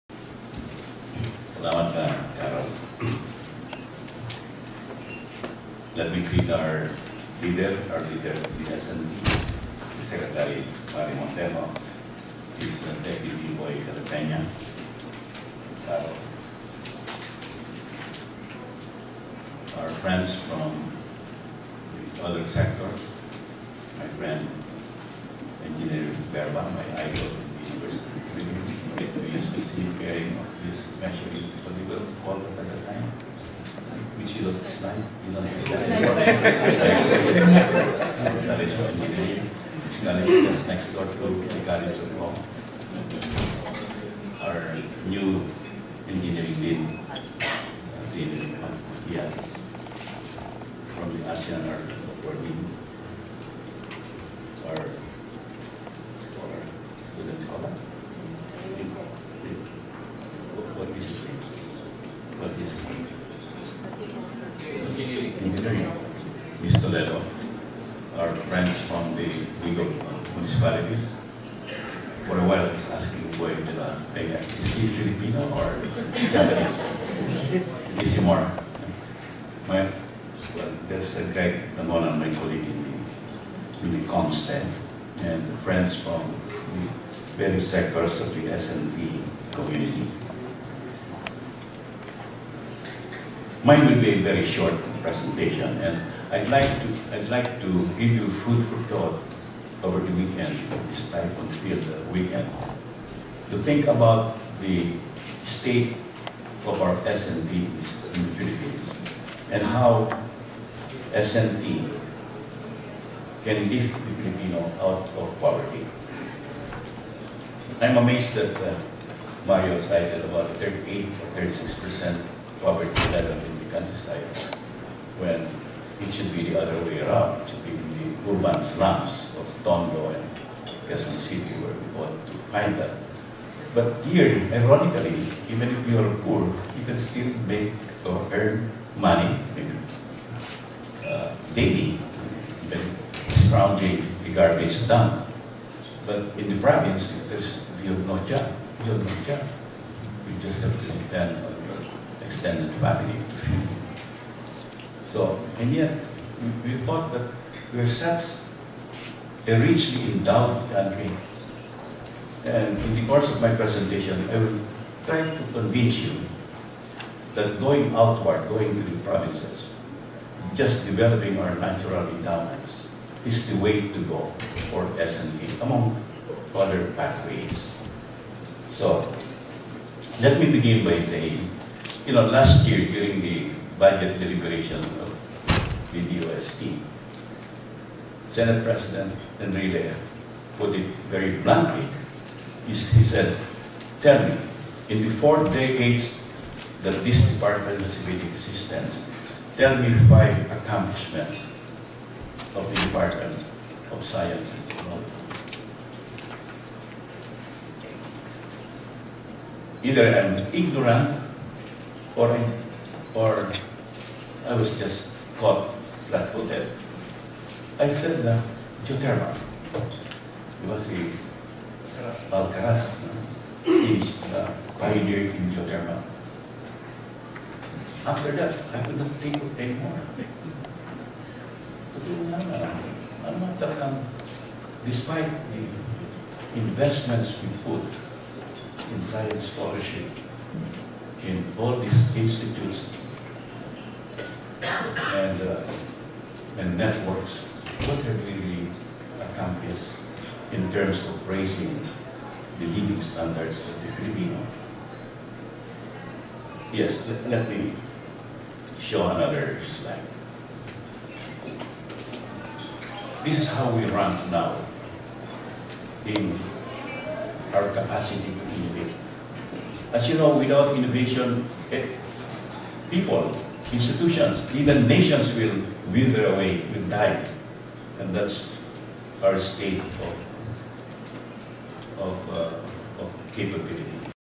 S&T Stakeholders Summit (1 of 9) - July 29, 2011
Keynote speech of Philippine Senator Edgardo J. Angara during the DOST National Science and Technology Week celebration at the SMX Covention Center on July 29, 2011.